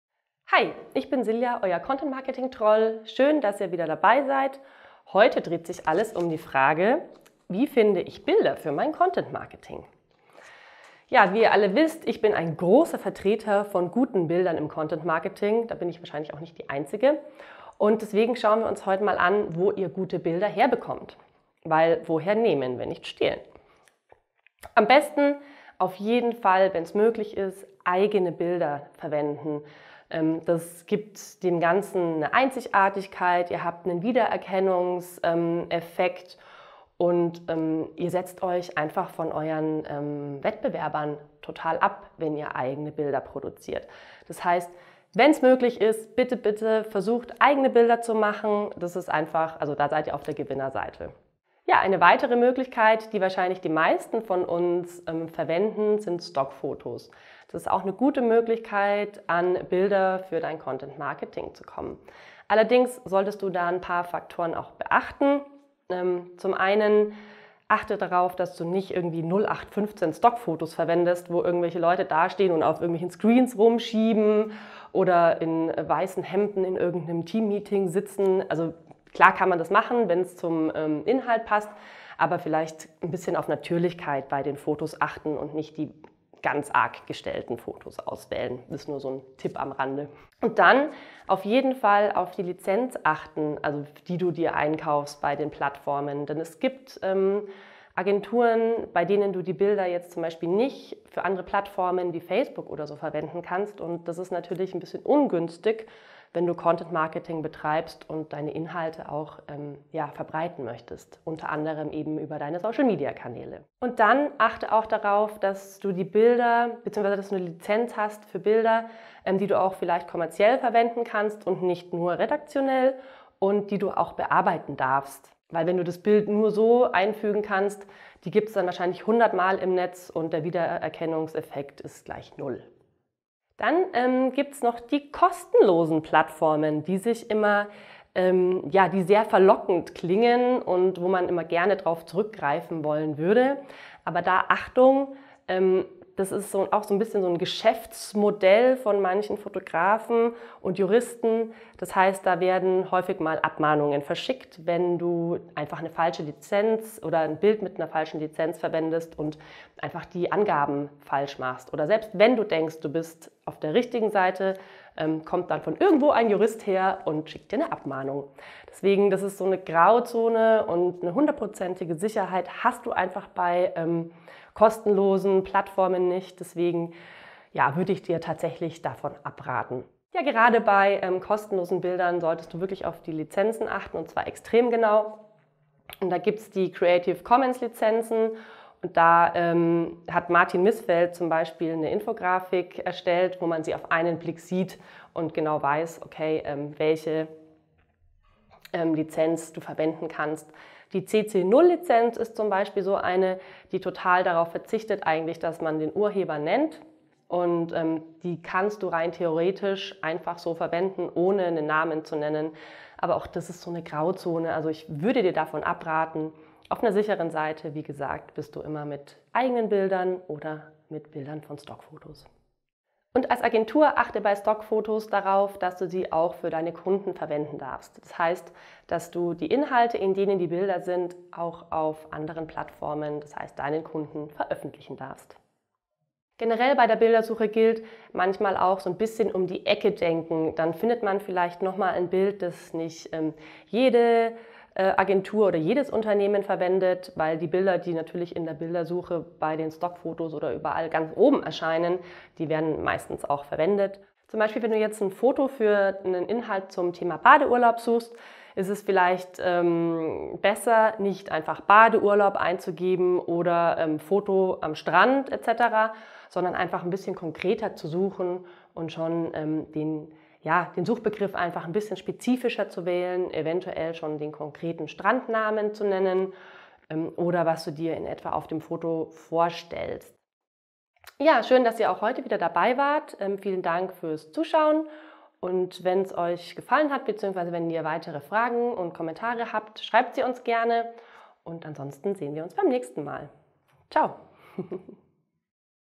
Bildersuche: Wie finde ich Bilder für mein Content Marketing | Seokratie
Du willst passende und vor allem einzigartige Bilder für deine Content-Marketing-Inhalte? Unsere Content-Marketing-Expertin Silja listet Dir auf, welche Möglichkeiten Du für Deine Bildersuche und Erstellung hast und welche davon die beste Option ist. Außerdem erklärt sie Dir, auf was Du unbedingt achten solltest.

Hier geht’s zu "Creative Commons Lizenzen" von Martin Mißfeldt

Für mehr hilfreiche Tipps zum Thema Content Marketing schau gerne auf dem Seokratie-Blog vorbei:

Dein Seokratie-Team